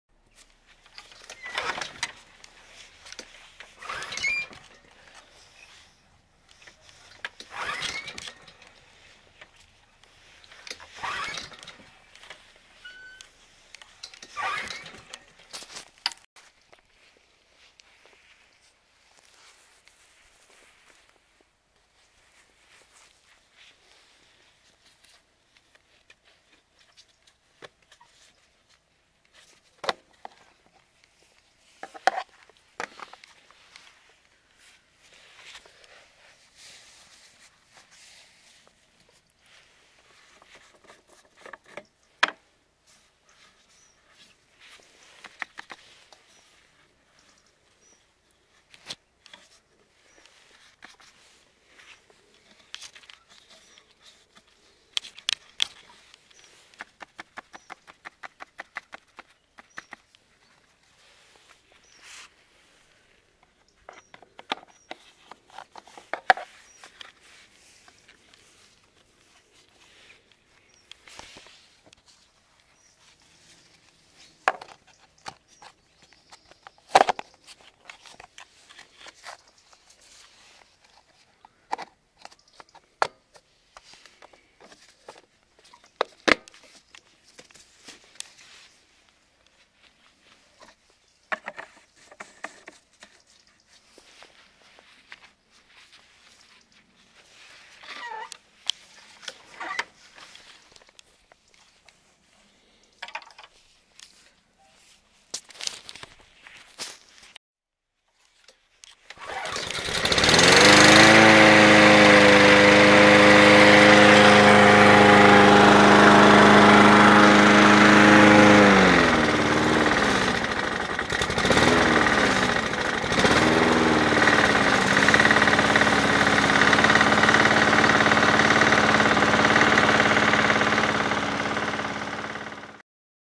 Comment démarrer une tondeuse à gazon qui refuse de démarrer après l'hiver ou un arrêt prolongé .
Démarrer une tondeuse à gazon qui refuse de démarrer après  l'hiver ou un arrêt prolongé  .
vous dévissez le bougon du réservoir , vous prenez une peu d'essence et vous imbibé un peu d'essence sur le filtre à air et votre tondeuse va démarrer sans difficulté sincèrement vous serez surpris !
Attention bien remettre le filtre à air dans son emplacement d'origine